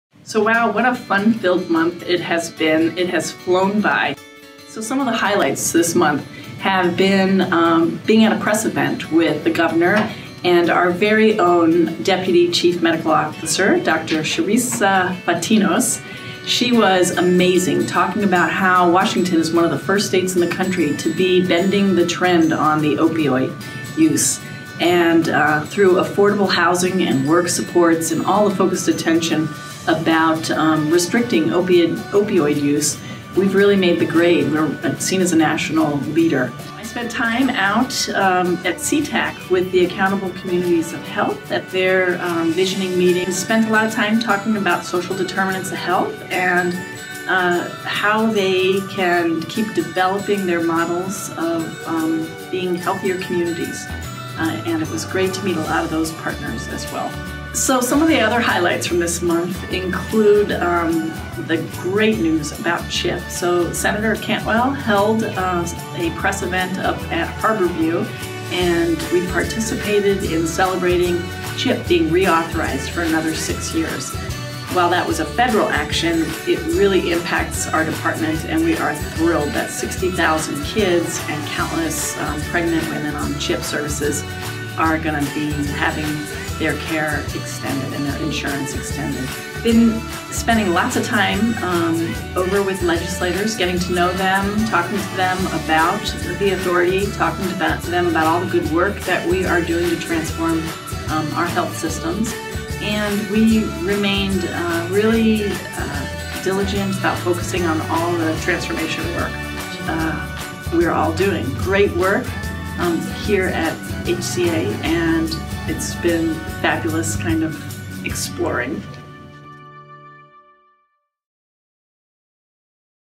Director's January 2018 update
What went on during Sue's first month as HCA's Director? Get the January news about meeting legislators, the Governor's opioid epidemic response, accountable communities of health, and re-authorization of CHIP funding.